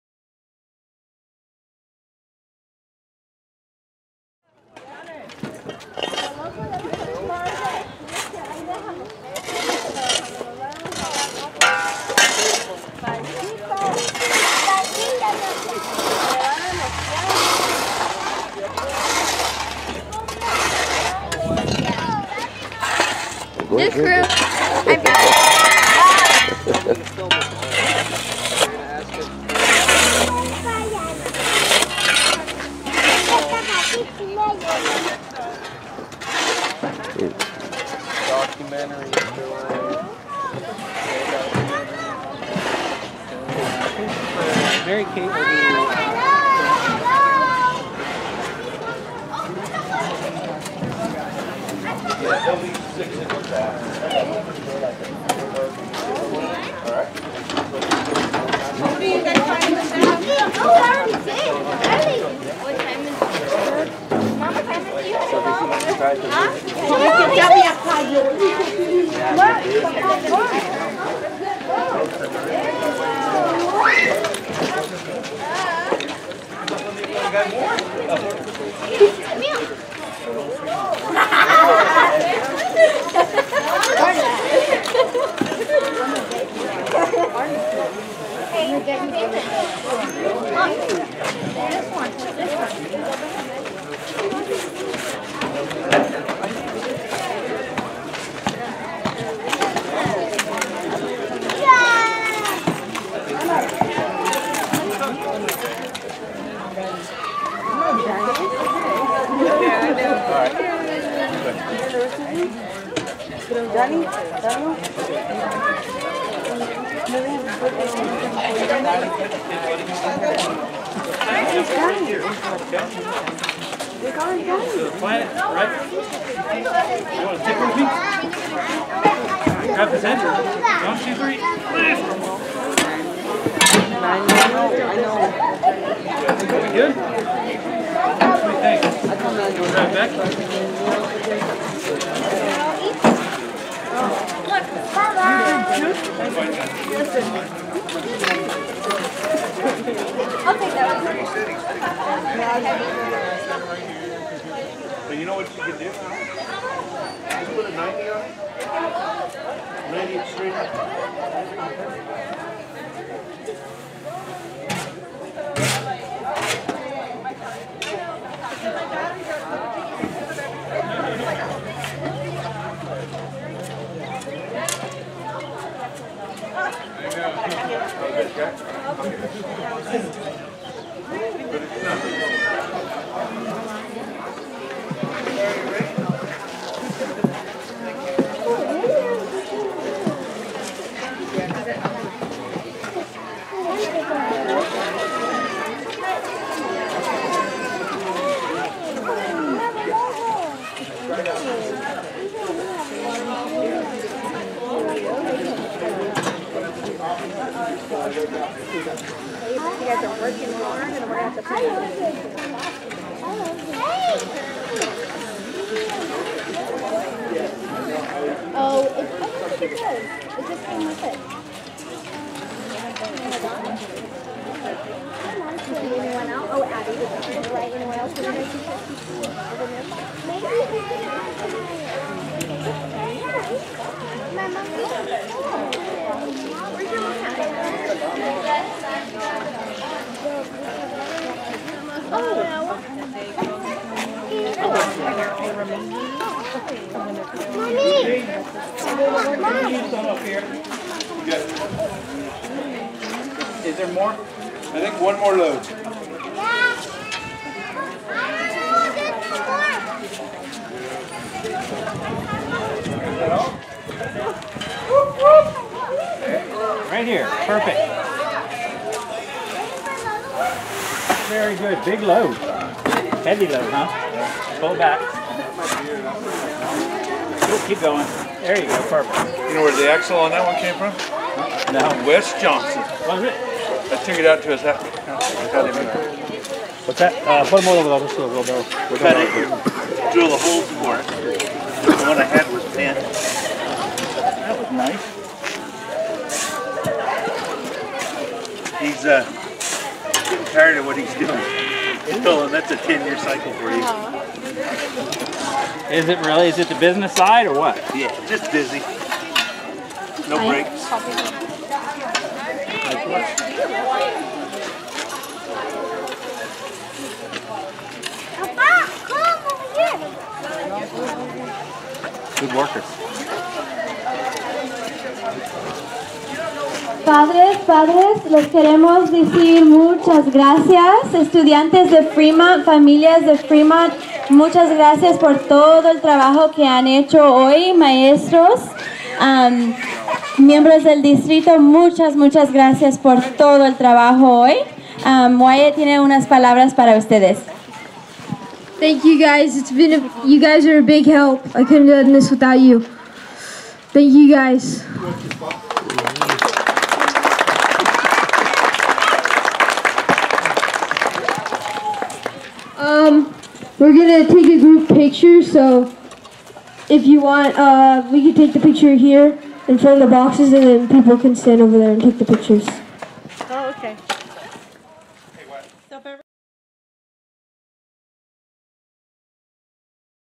2018 January 27 Fremont School Garden Project
What an awesome turnout by our community - coming together to build this vegetable/fruit garden for the Fremont students!!  Although short, this snippet of a video gives you a feel for how we built this foundation for a plethora of tasty food to come!